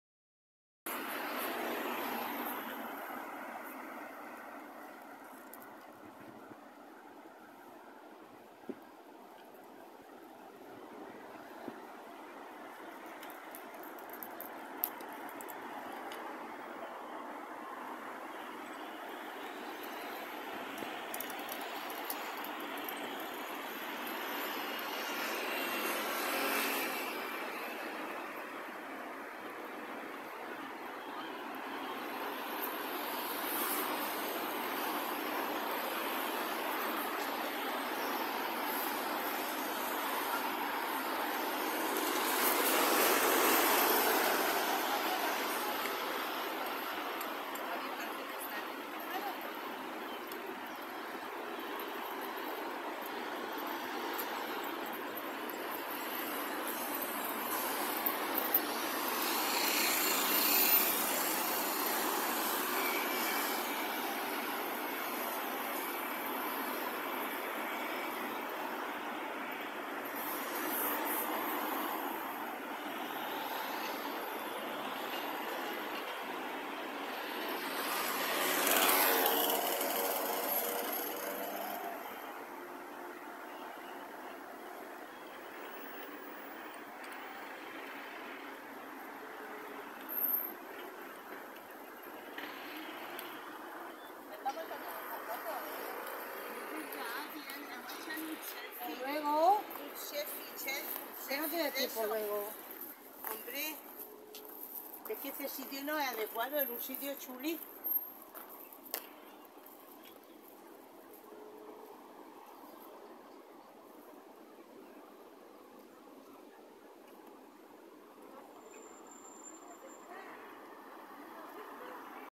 BENIDORM MOSQUE - VIEW OF BENIDORM FROM A MOBILITY SCOOTER
Benidorm has a lot to offer, with its nightlife, many beaches and its many bars and restaraunts for great value food and drink.  Join me on my mobility scooter for a different view of Benidorm.